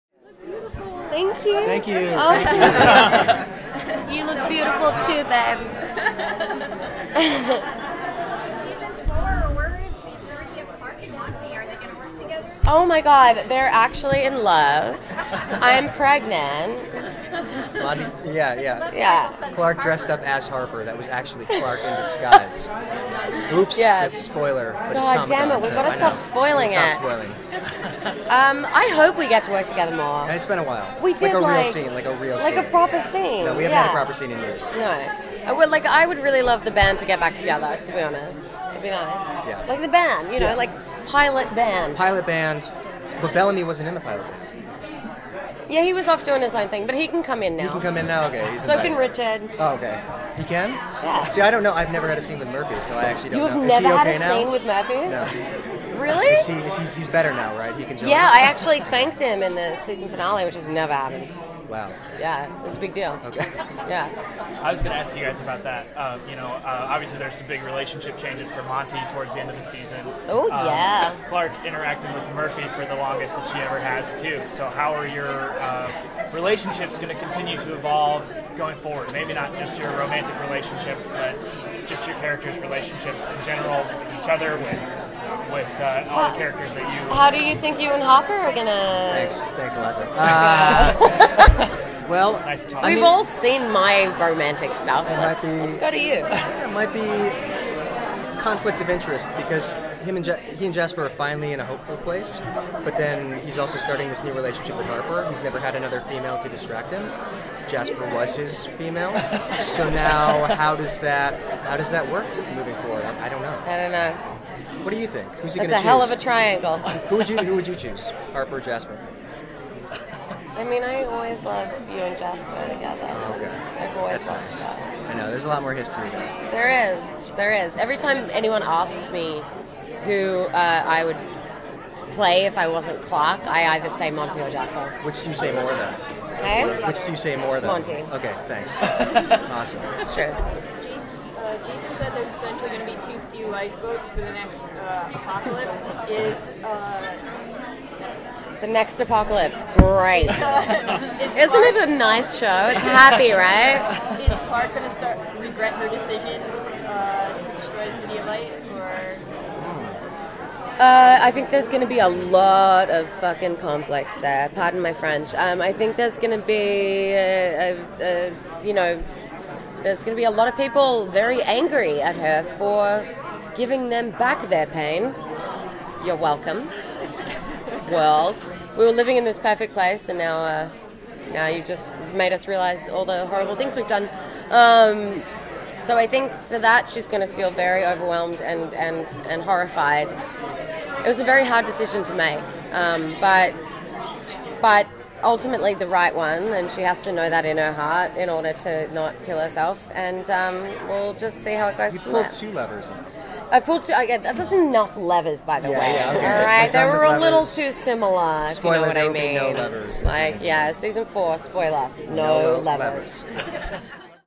THE 100 SEASON 4 - Eliza Taylor & Christopher Larkin Interview on What To Expect for Clarke & Monty
Eliza Taylor & Christopher Larkin interview on what we can expect for Clarke & Monty on THE 100 SEASON 4.

If you like this video, please make sure to click the like button and share this with all your friends.